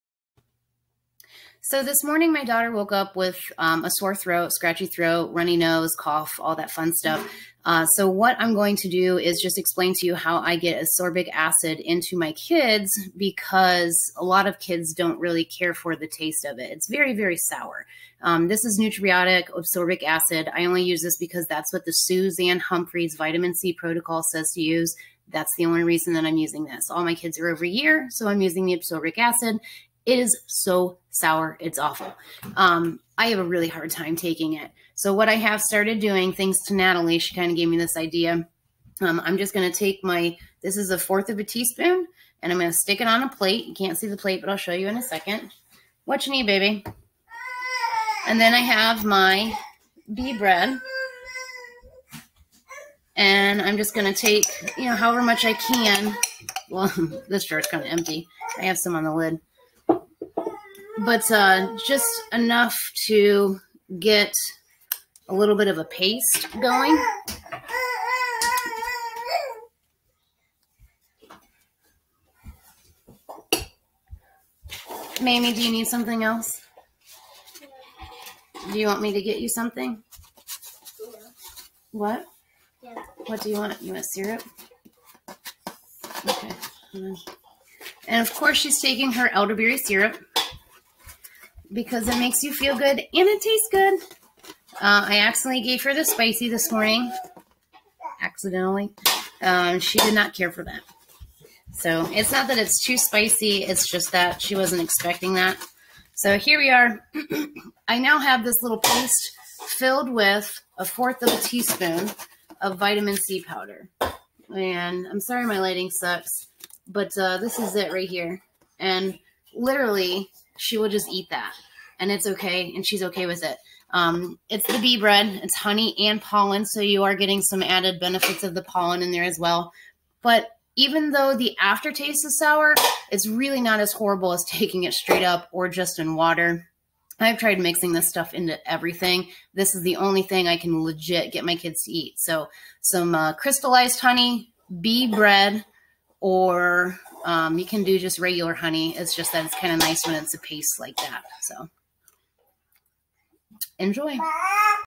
How we take Ascorbic Acid in raw honey
How I can get my kids to eat ascorbic acid.  We mix it into bee bread which is honey and pollen but any raw honey will do, crystallized the better!